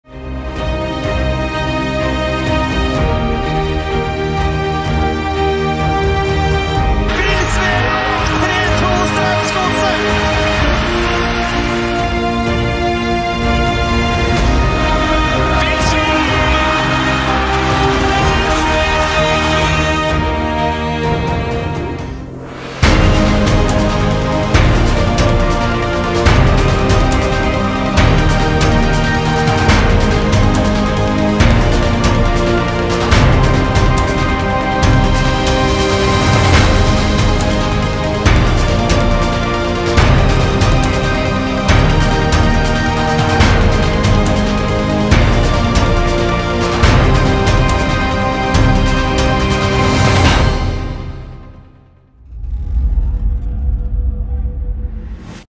𝐕𝐈𝐋𝐒𝐕𝐈𝐊 𝟐𝟎𝟐𝟑
Lars Vilsvik har spilt 355 kamper og tilbrakt hele sin profesjonelle karriere i Strømsgodset. Nå har han forlenget kontrakten med klubben med ytterligere to år.